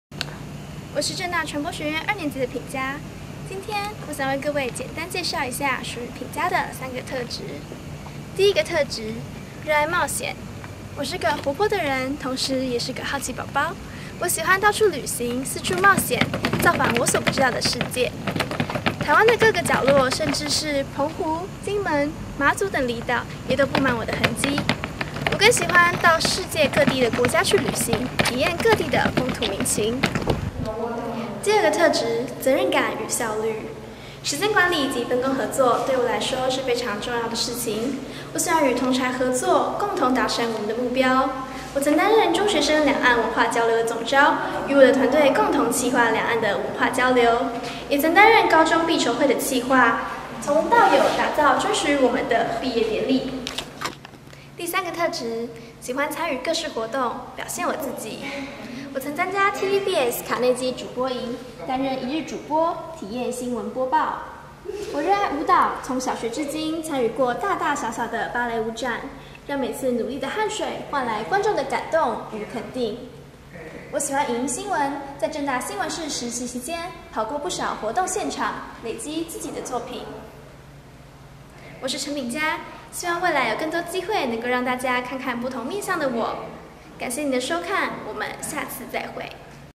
品嘉的三個特質【課堂作業】
一天內產出的自介影片
超趕超瘋狂XD
謝謝可愛的莉庭攝影師
溫暖的串場人員 卓希、軒筠